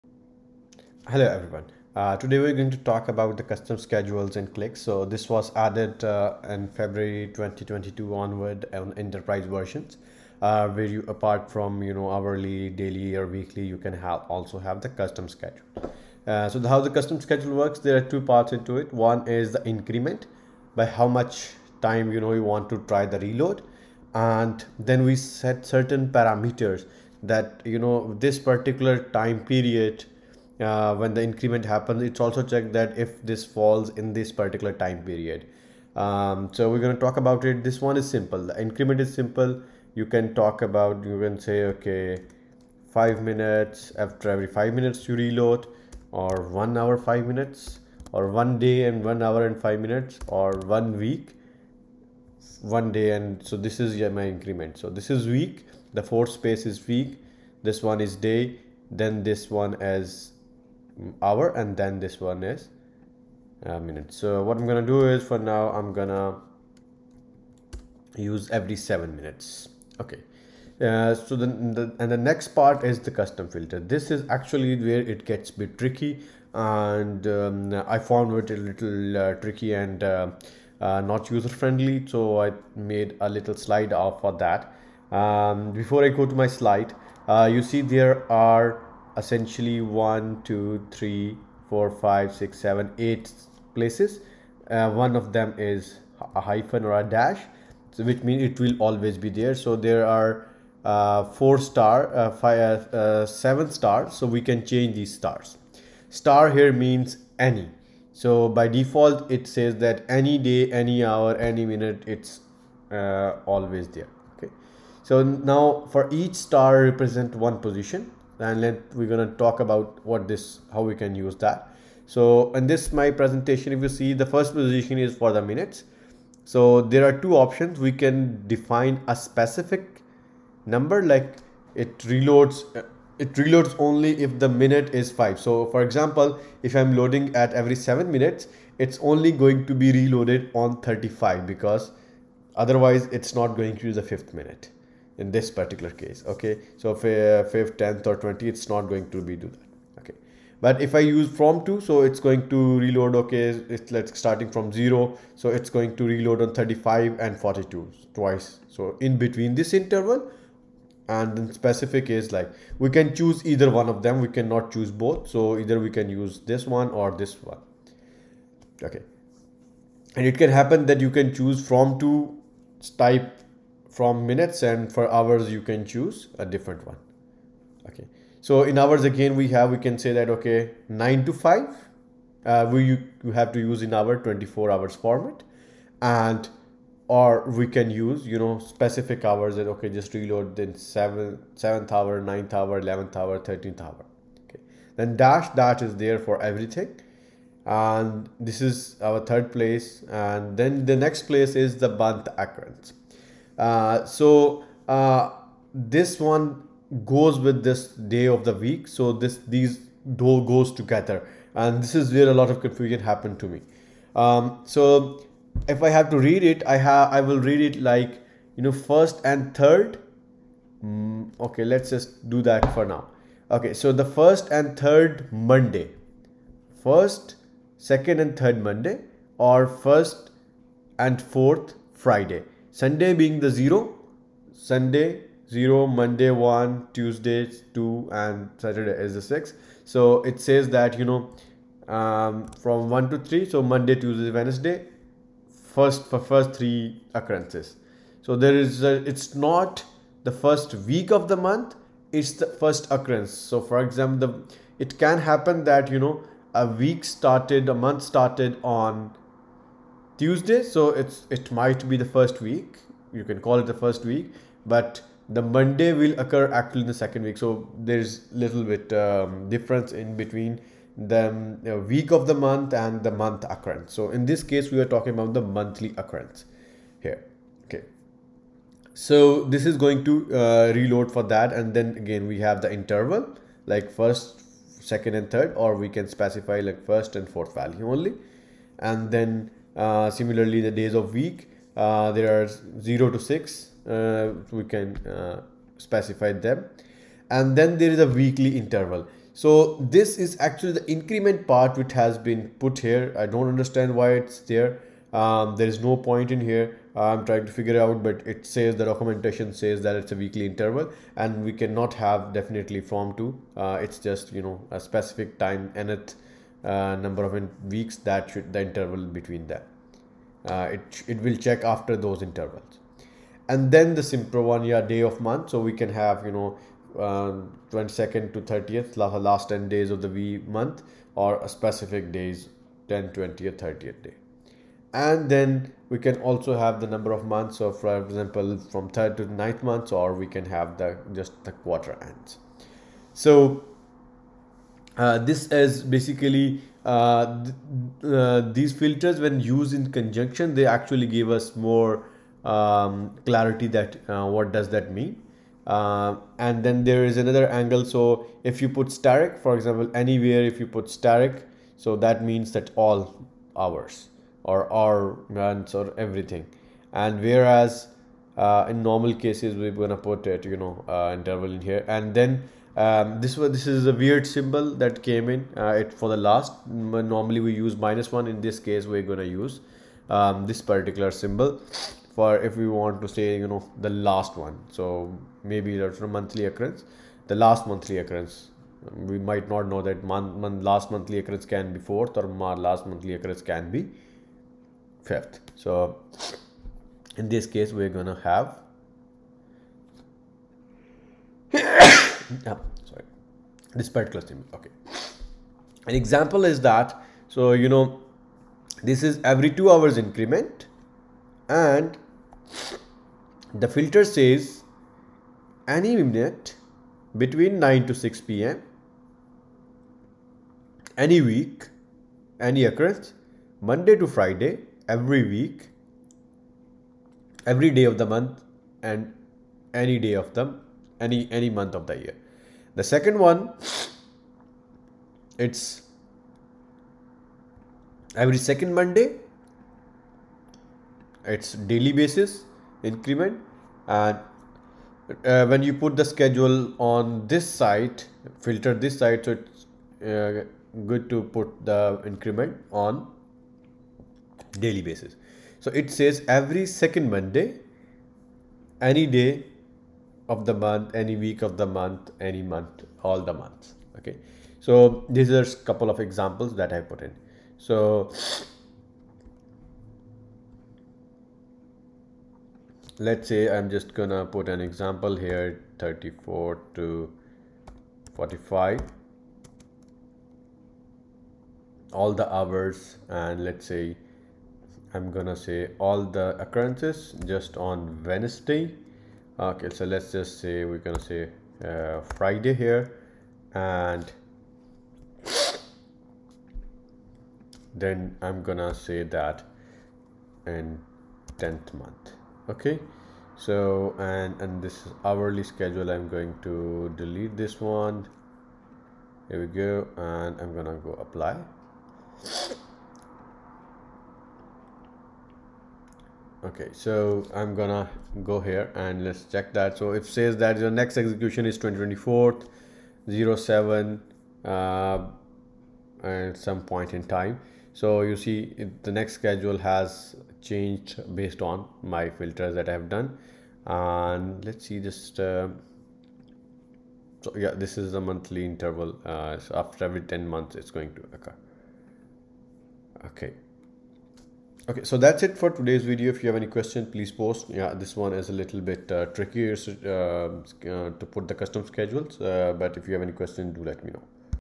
Qlik Sense QMC Custom Scheduling
Sometimes, I come across kind of schedules that are not standard or customized. One example is load in working hours only on Weekday.  Then either you have to set up multiple schedules or you can use QMC custom schedule option. In this video, I have decoded the Filter mystery.